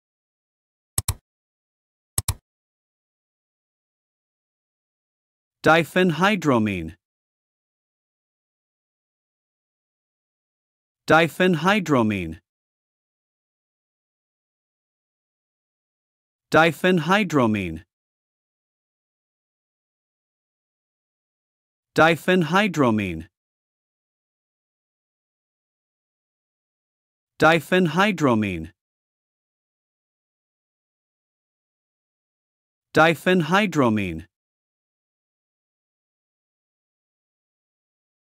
How to pronounce Diphenhydramine in English correctly
This video shows you how to say (Diphenhydramine). Listen and learn the English pronunciation of Diphenhydramine.

What is diphenhydramine?
Diphenhydramine is an antihistamine that reduces the effects of natural chemical histamine in the body. Histamine can produce symptoms of sneezing, itching, watery eyes, and runny nose.

Diphenhydramine is used to treat sneezing, runny nose, watery eyes, hives, skin rash, itching, and other cold or allergy symptoms.

Diphenhydramine is also used to treat motion sickness, to induce sleep, and to treat certain symptoms of Parkinson's disease.